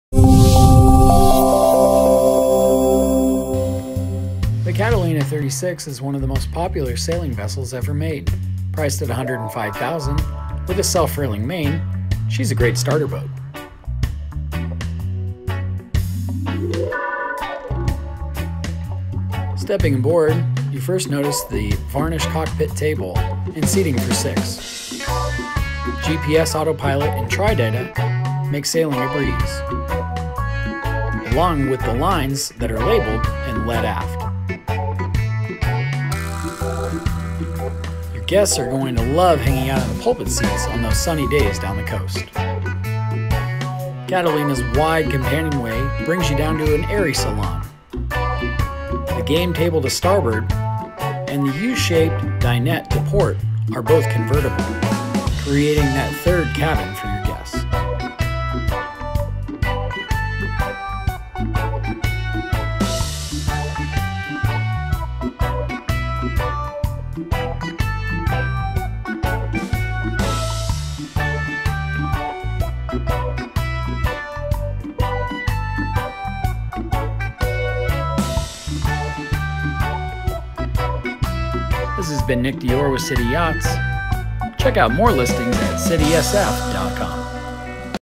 SOLD!! 2002 Catalina 36 MKII - $79,000 - City Yachts Listing. Recently Reduced Owner wants an offer
City Yachts
Christine Kaplan